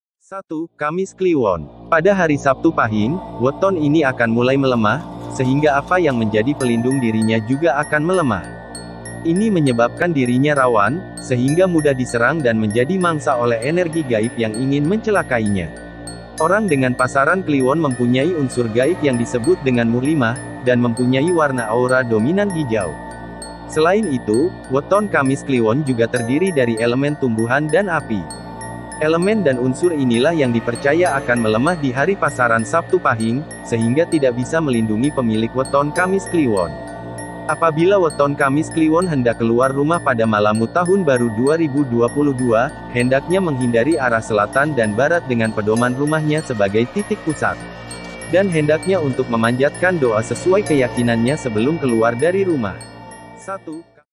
Weton Berbahaya di pergantian tahun 2021 - 2022
Weton Berbahaya Menurut Ramalan Jawa Boleh percaya / tidak.